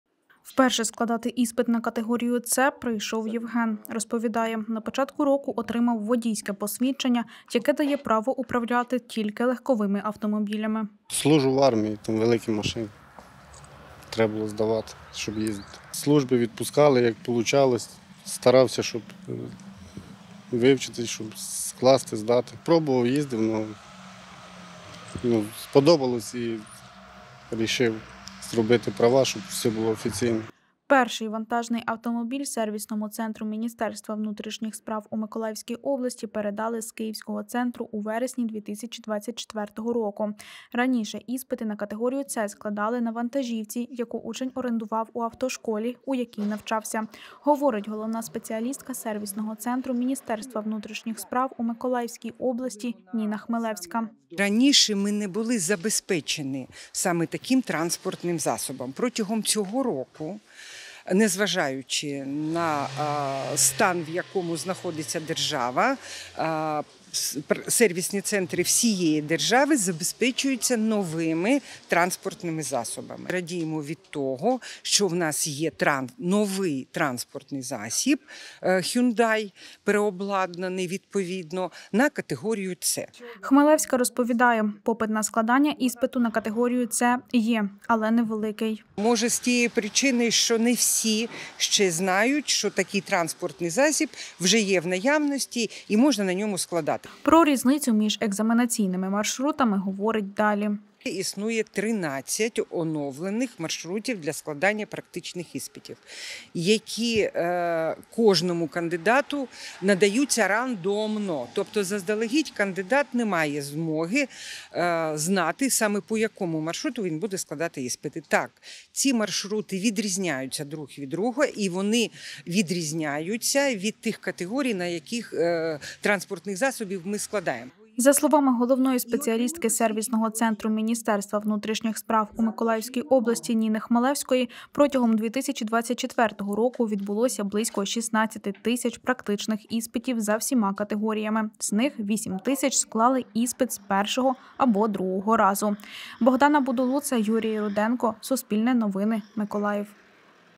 Як у Миколаєві складають іспити категорії "С"
У вересні 2024 року Миколаївський сервісний центр МВС отримав нову вантажівку. Тепер для складання іспитів категорії "С" не потрібно орендувати вантажний автомобіль в автошколи.